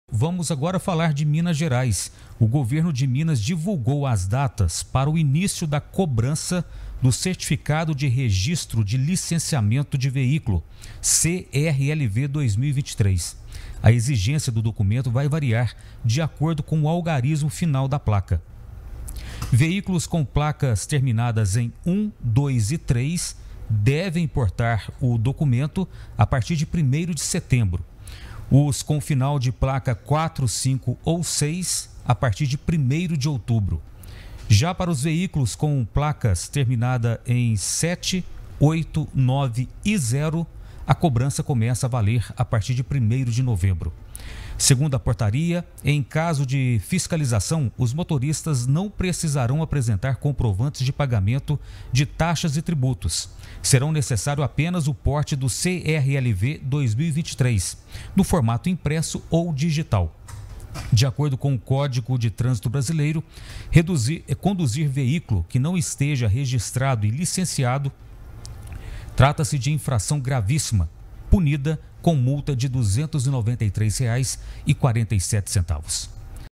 CRLV 2023 SERÁ EXIGIDO A PARTIR DE SETEMBRO EM MINAS GERAIS
| MINAS GERAIS |

O GOVERNO DE MINAS GERAIS DIVULGOU AS DATAS PARA O INÍCIO DA COBRANÇA DO CERTIFICADO DE REGISTRO E LICENCIAMENTO DE VEÍCULO (CRLV) 2023. A EXIGÊNCIA DO DOCUMENTO VAI VARIAR DE ACORDO COM O ALGARISMO FINAL DAS PLACAS. VEJA AS DATAS: